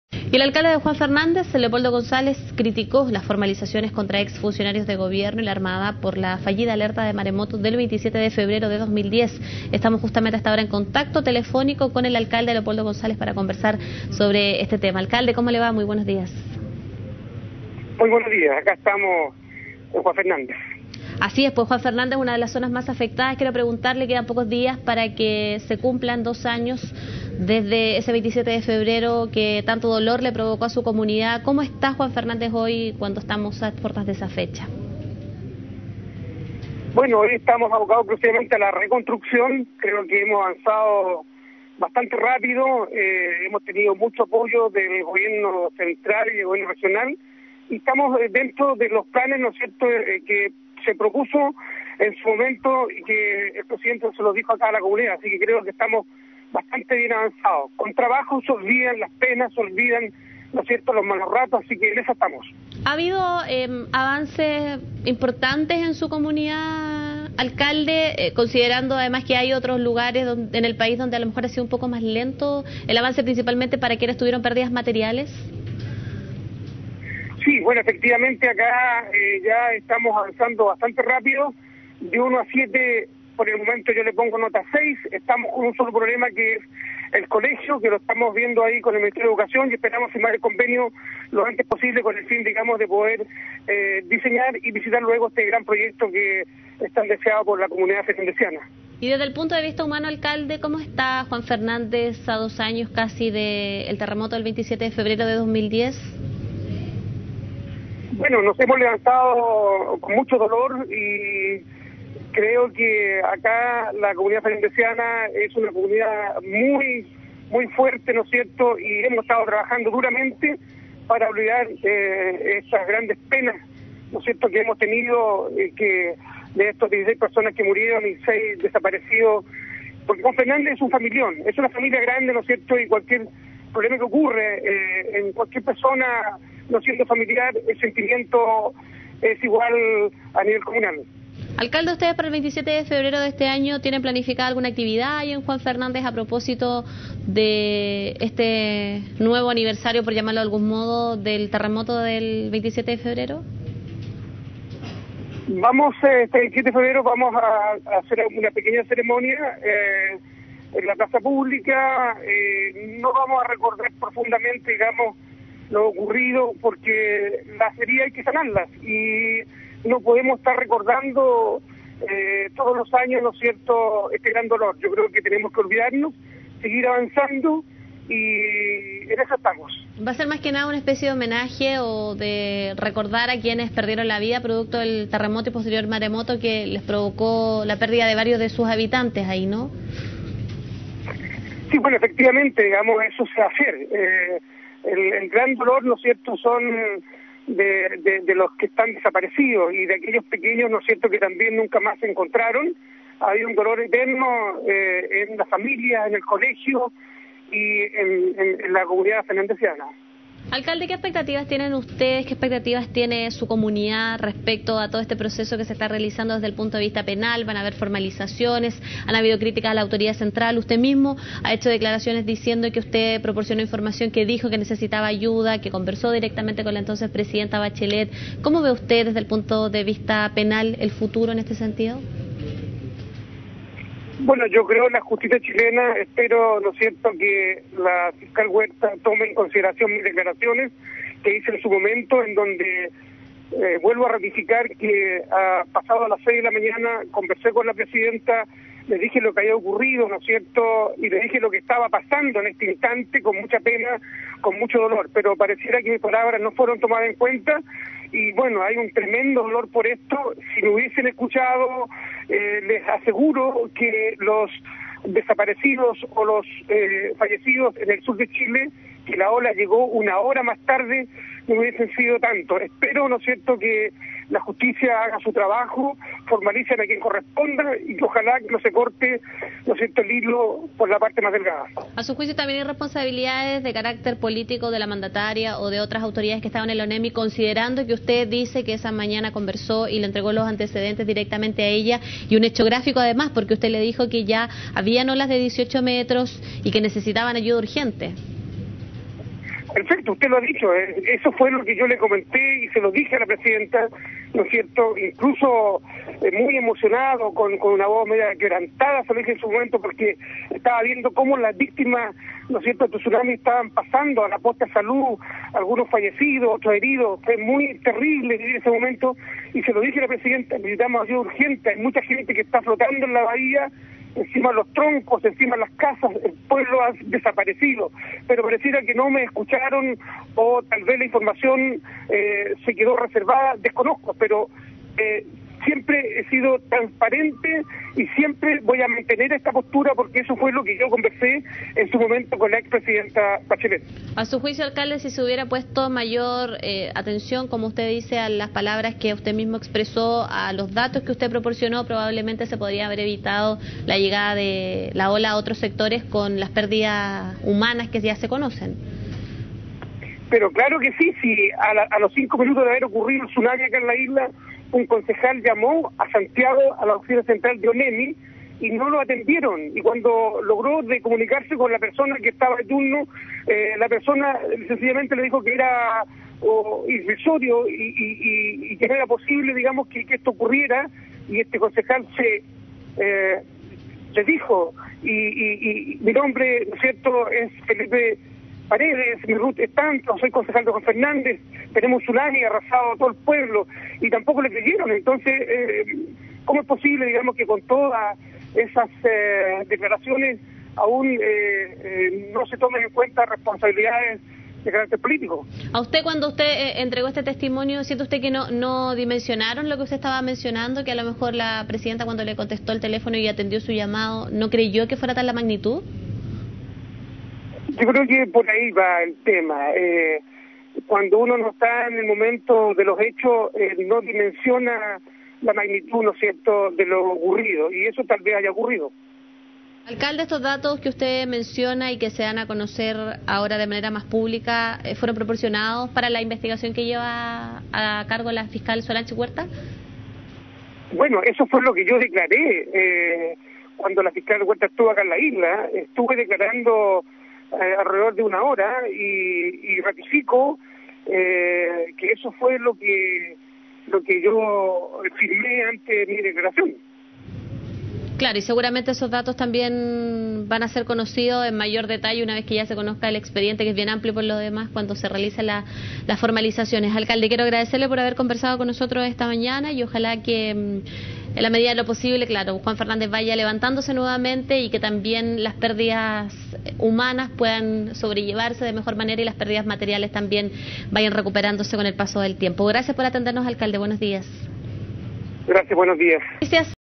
Alcalde de Juan Fernández criticó formalizaciones contra ex funcionarios de Gobierno
A pocos días de que se cumplan dos años del terremoto 8.8, el Alcalde de Juan Fernández, Leopoldo González, emplazó a la Presidenta Michelle Bachellet como responsable de la mala gestión durante el  27 de febrero del 2010